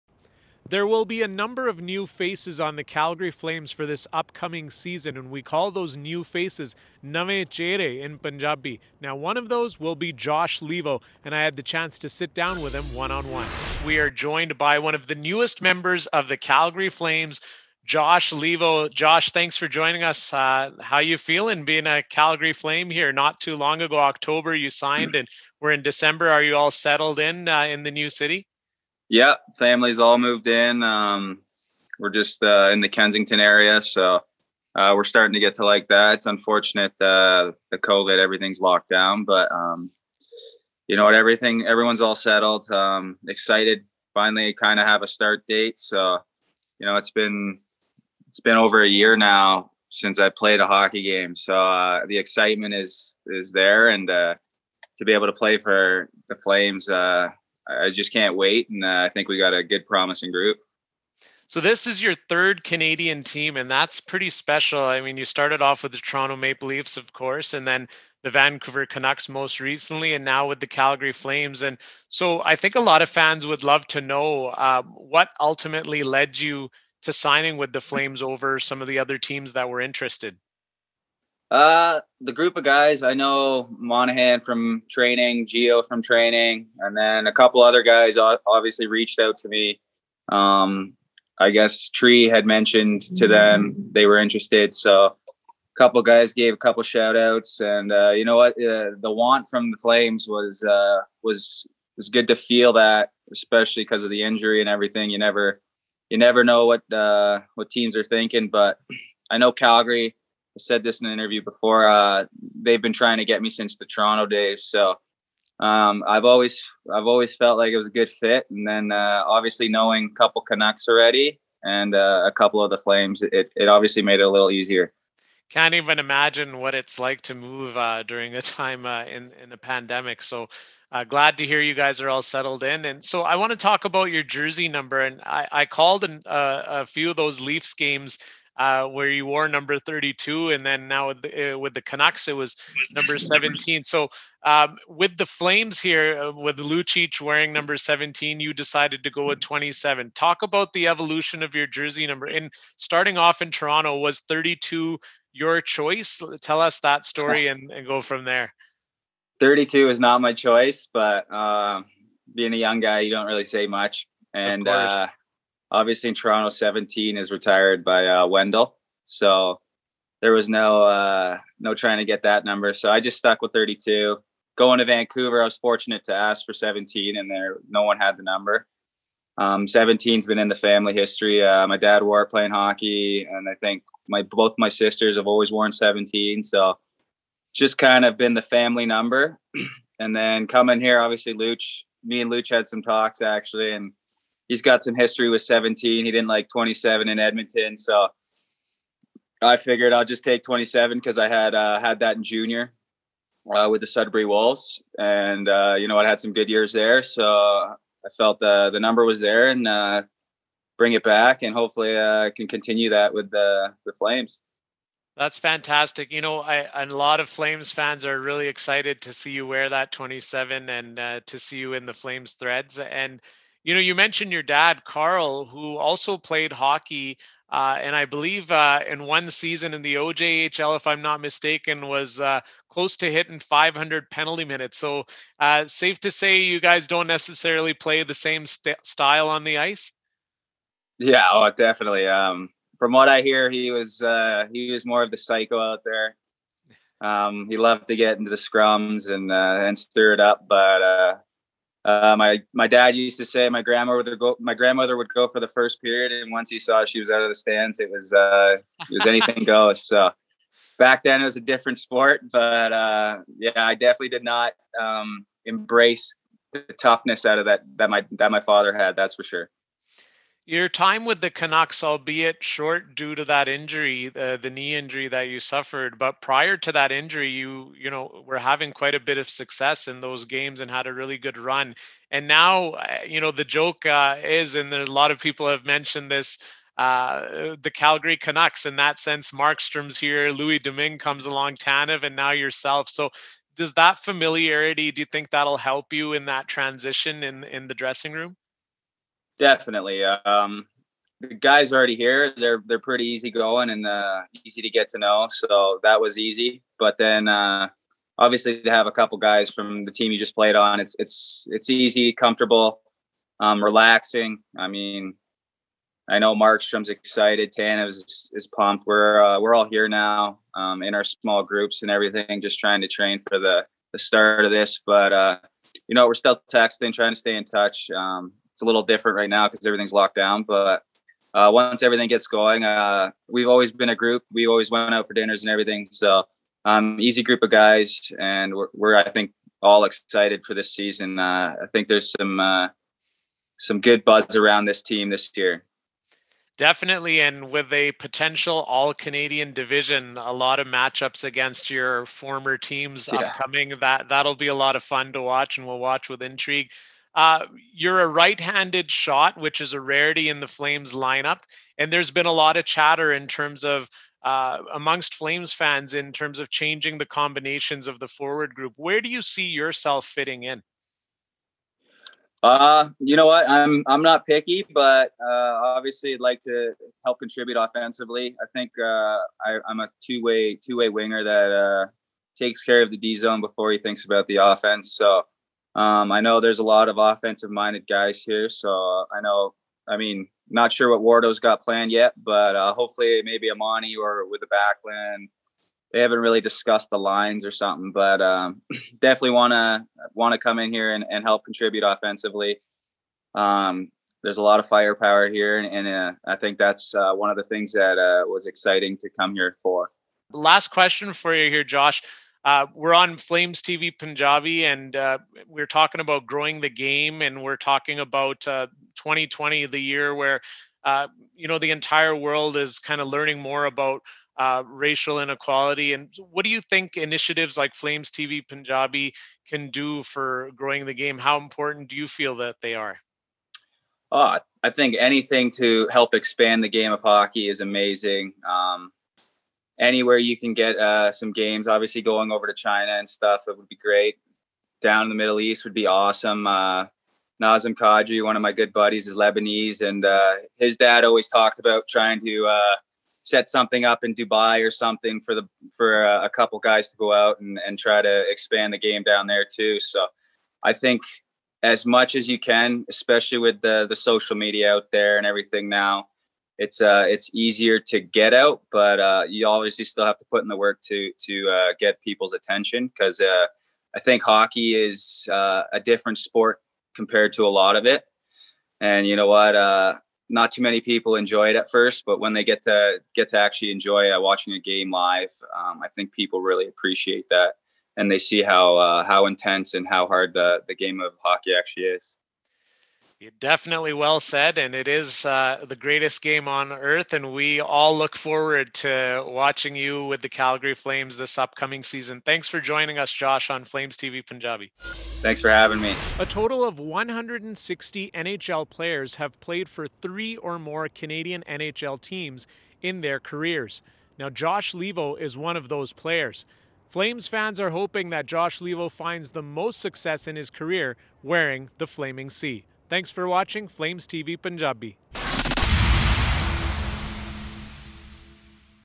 Flames TV Punjabi - Harnarayan Singh with Josh Leivo
Harnarayan catches up with one of the newest Flames to get his thoughts on the upcoming season.